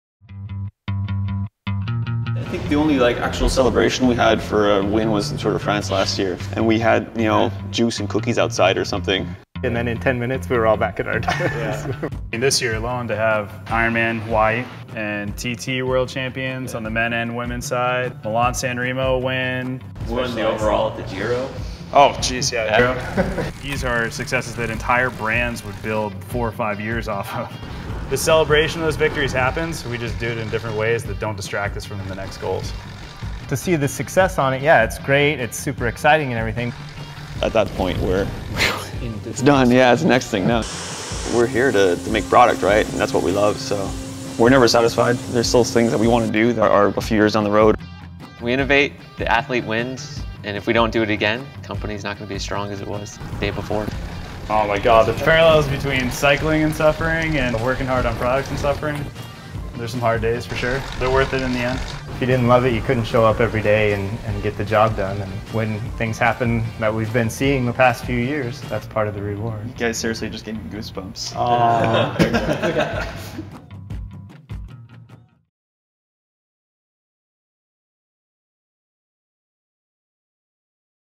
I Am Specialized Road R&D
Behind the teams that have won the Tour de France, the most prestigious Classics, and the world championships, is a team of fanatical engineers that create the highest performance road racing bicycles in the world.